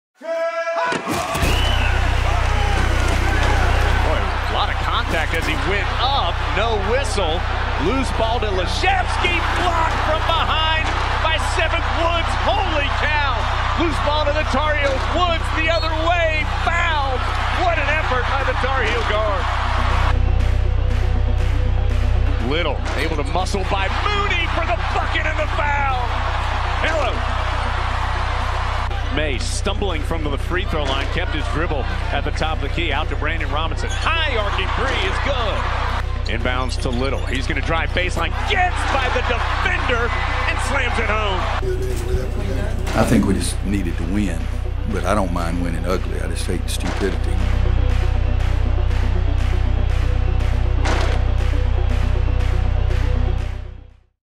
Listen to the top calls from UNC's win over Notre Dame
It wasn't pretty, but UNC got a critical conference win over Notre Dame. Luke Maye and Coby White combined for 31 of UNC's 75 points and Seventh Woods fired up the crowd with a highlight-worthy block in the second half. Listen to the top calls from the game by Tar Heels Sports Network play-by-play announcer Jones Angell.

for more UNC basketball coverage.